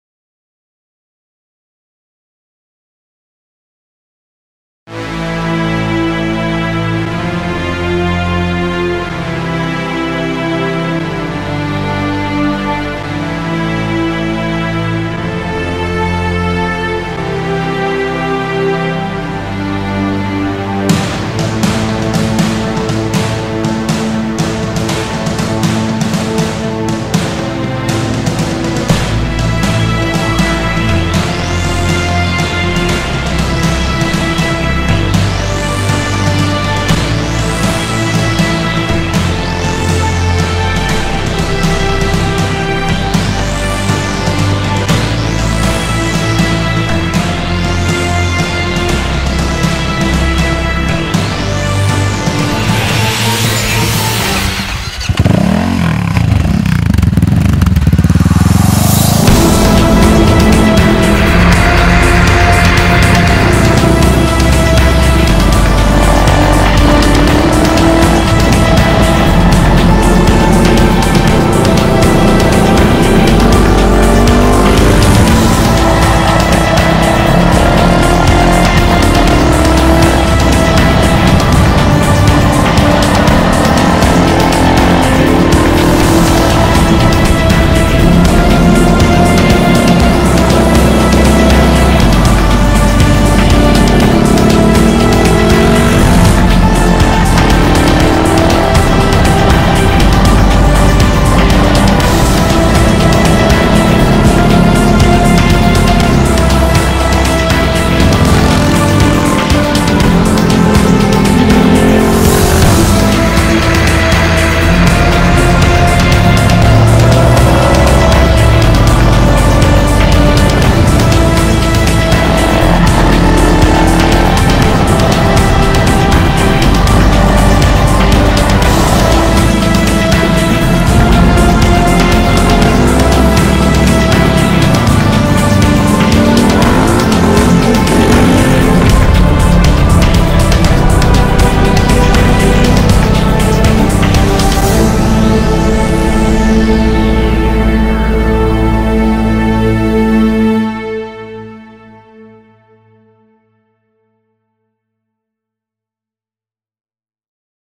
BEAUTIFUL HAND BUILT BRITISH MOTORCYCLE | First Ride
CCM are are a British motorcycle manufacturer with deep roots in the off road motorcycle sector.  We are proud to be working with them to produce the media for their new Spitfire range of hand built motorcycles including the original Spitfire, Spitfire Scrambler and Spitfire Cafe racer. We joined them to film the first Spitfire to hit the road. To find out more about their stunning range